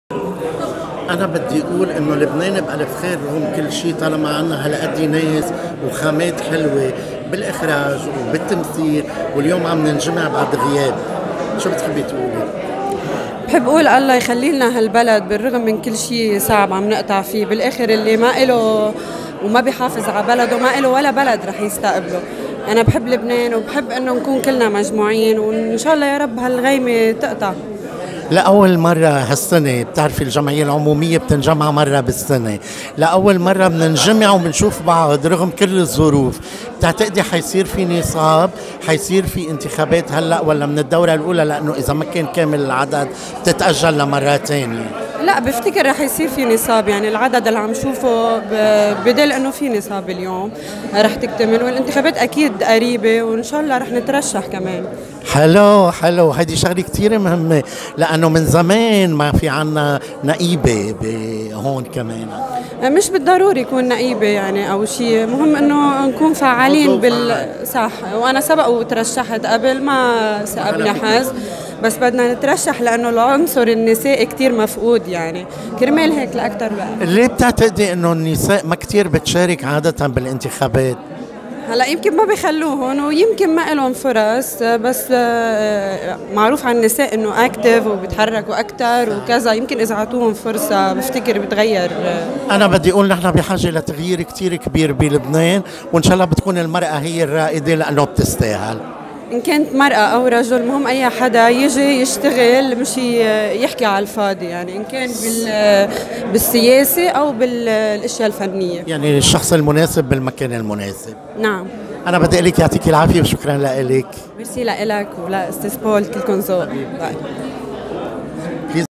المخرجة اللبنانية دلال عبد الله حول اجتماع الجمعية العمومية
المخرجة اللبنانية دلال عبد الله حول اجتماع الجمعية العمومية الذي جرى في مسرح شمس  .. الطيونة .. واللقاء مع الأصدقاء الممثلين والفنانين بعد طول غياب